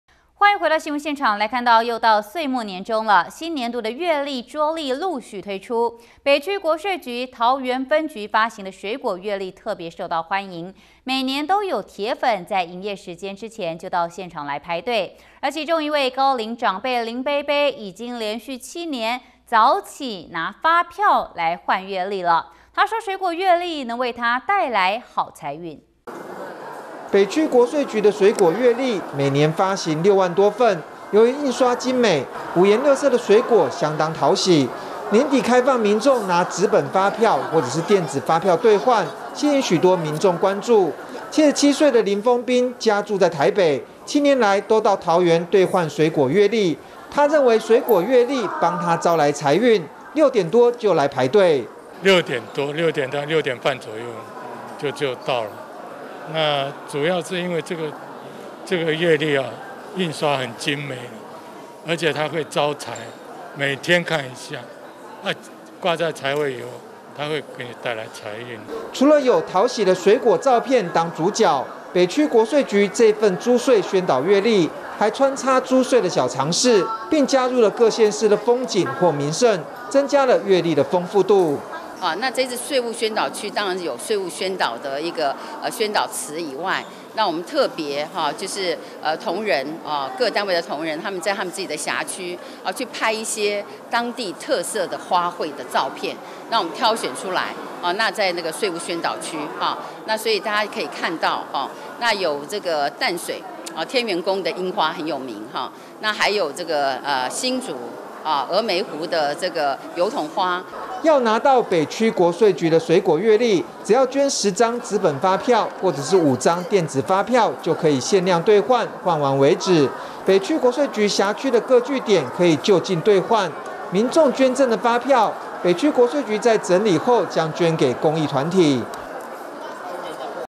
北區國稅局月曆搶手 鐵粉民眾早起排隊│T-NEWS聯播網
又到歲末年終，新年度的月曆、桌曆陸續推出。北區國稅局發行的水果月曆特別受歡迎，每年有鐵粉在營業時間之前就到現場排隊；其中一位高齡長輩林伯伯，已經連續七年起早拿發票來換月曆，他說水果月曆能為他帶來好財運！